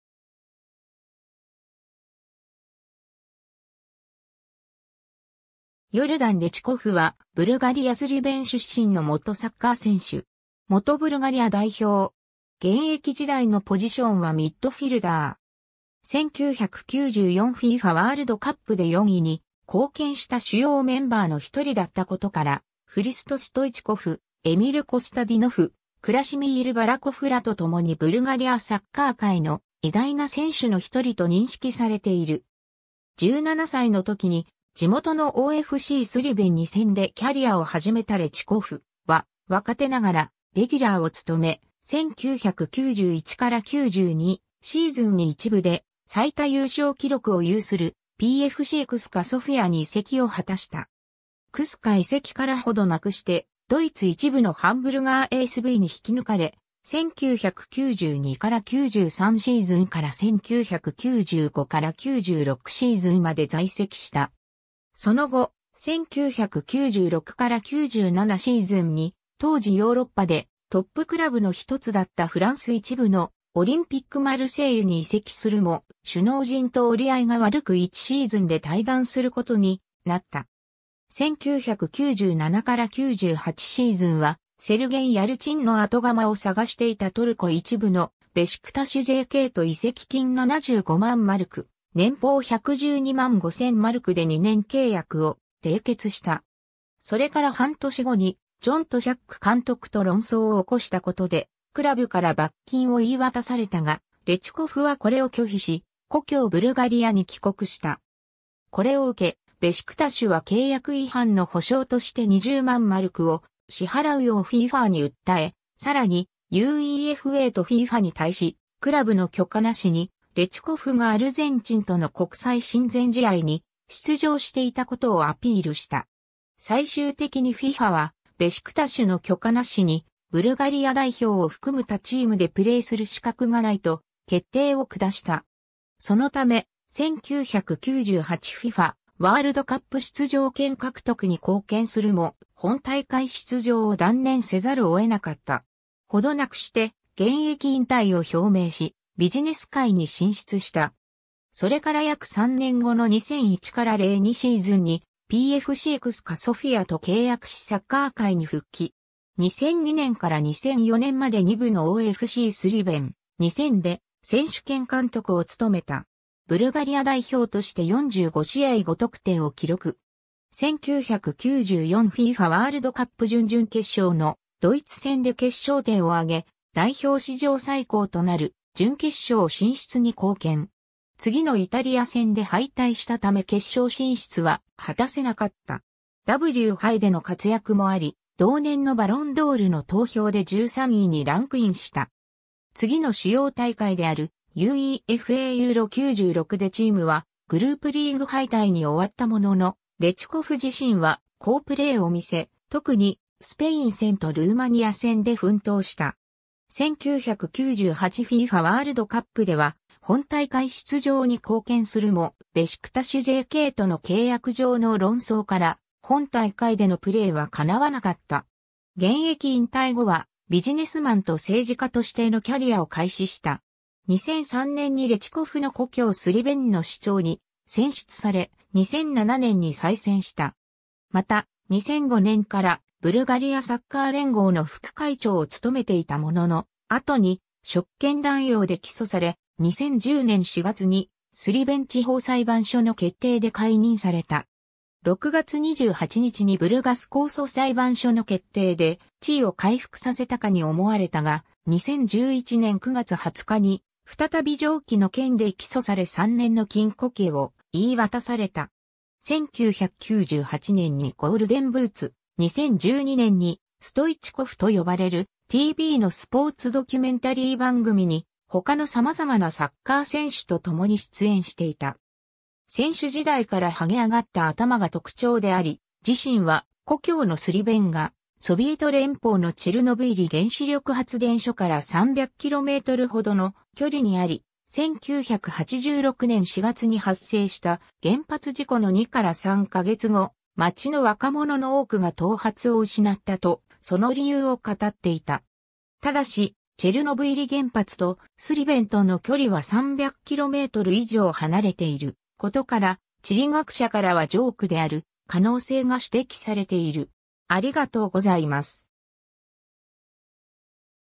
ヨルダン・レチコフ
ヨルダン・レチコフ（ブルガリア語: Йордан Лечков, ラテン文字転写: Yordan Letchkov, 1967年7月9日- ）は、ブルガリア・スリヴェン出身の元サッカー選手。
元ブルガリア代表。
現役時代のポジションはミッドフィルダー。
 1994 FIFAワールドカップで4位に貢献した主要メンバーの1人だったことから、フリスト・ストイチコフ、エミル・コスタディノフ、クラシミール・バラコフらと共にブルガリアサッカー界の偉大な選手の1人と認識されている。
 17歳の時に地元のOFCスリヴェン2000でキャリアを始めたレチコフは、若手ながらレギュラーを務め、1991-92シーズンに1部で最多優勝記録を有するPFC CSKAソフィアに移籍を果たした。
CSKA移籍から程なくしてドイツ1部のハンブルガーSVに引きぬかれ、1992-93シーズンから1995-96シーズンまで在籍した。
その後、1996-97シーズンに当時ヨーロッパでトップクラブの1つだったフランス1部のオリンピック・マルセイユに移籍するも、首脳陣と折り合いが悪く1シーズンで退団することになった。
 1997-98シーズンは、セルゲン・ヤルチンの後釜を探していたトルコ1部のベシクタシュJKと移籍金75万マルク・年俸112万5000マルクで2年契約を締結した。
それから半年後にジョン・トシャック監督と論争をおこしたことでクラブから罰金を言い渡されたが、レチコフはこれを拒否し故郷ブルガリアに帰国した。
これを受け、ベシクタシュは契約違反の補償として20万マルクを支払うようFIFAに訴え、さらにUEFAとFIFAに対し、クラブの許可なしにレチコフがアルゼンチンとの国際親善試合に出場していたことをアピールした。
最終的にFIFAはベシクタシュの許可なしにブルガリア代表を含む他チームでプレーする資格がないと決定を下した。
そのため、1998 FIFAワールドカップ出場権獲得に貢献するも、本大会出場を断念せざるを得なかった。
程なくして現役引退を表明し、ビジネス界に進出した。
 それから約3年後の2001-02シーズンにPFC CSKAソフィアと契約しサッカー界に復帰。
2002年から2004年まで2部のOFCスリヴェン2000で選手兼監督を務めた。
 ブルガリア代表として45試合5得点を記録。
1994 FIFAワールドカップ準々決勝のドイツ戦で決勝点を挙げ、代表史上最高となる準決勝進出に貢献。
次のイタリア戦で敗退したため決勝進出は果たせなかった。
W杯での活躍もあり、同年のバロンドールの投票で13位にランクインした（ちなみに1位は、同胞のフリスト・ストイチコフだった）。
次の主要大会であるUEFA EURO '96でチームはグループリーグ敗退に終わったものの、レチコフ自身は好プレーをみせ、特にスペイン戦とルーマニア戦で奮闘した。
 1998 FIFAワールドカップでは本大会出場に貢献するも、ベシクタシュJKとの契約上の論争から本大会でのプレーは叶わなかった。
 現役引退後は、ビジネスマン（高級ホテルとサッカーアカデミーを所有）と政治家としてのキャリアを開始した。
2003年にレチコフの故郷スリヴェンの市長に選出され、2007年に再選した。
また、2005年からブルガリアサッカー連合の副会長を務めていたものの、後に職権濫用で起訴され、2010年4月にスリヴェン地方裁判所の決定で解任された。
6月28日にブルガス控訴裁判所の決定で地位を回復させたかに思われたが、2011年9月20日に再び上記の件で起訴され3年の禁固刑を言い渡された。
 1998年に"ゴールデンブーツ"、2012年に"ストイチコフ"と呼ばれるTVのスポーツドキュメンタリー番組に他の様々なサッカー選手と共に出演していた。
 選手時代から禿げ上がった頭が特徴であり、自身は「故郷のスリヴェンがソビエト連邦のチェルノブイリ原子力発電所から300キロメートルほどの距離にあり、1986年4月に発生した原発事故の2から3か月後、街の若者の多くが頭髪を失った」と、その理由を語っていた。
ただし、チェルノブイリ原発とスリヴェンとの距離は300キロメートル以上離れ...